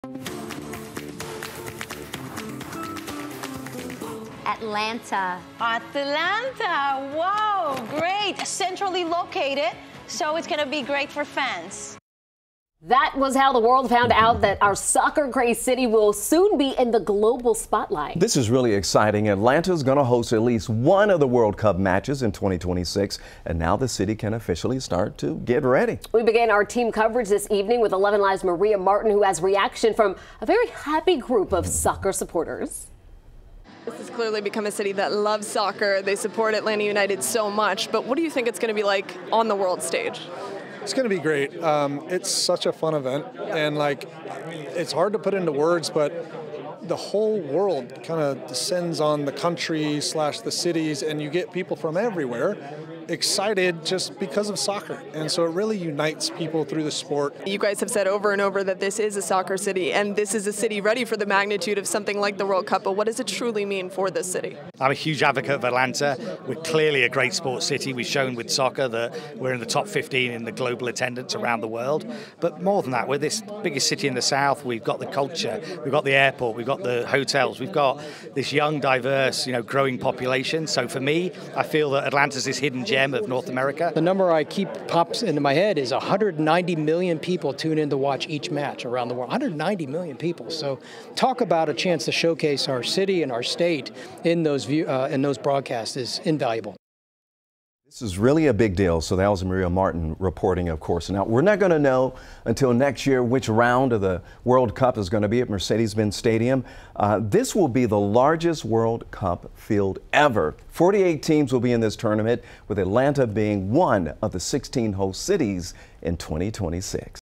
Atlanta tapped as one of 16 host cities for World Cup
Atlanta is swelling with pride as it proves it is a soccer city.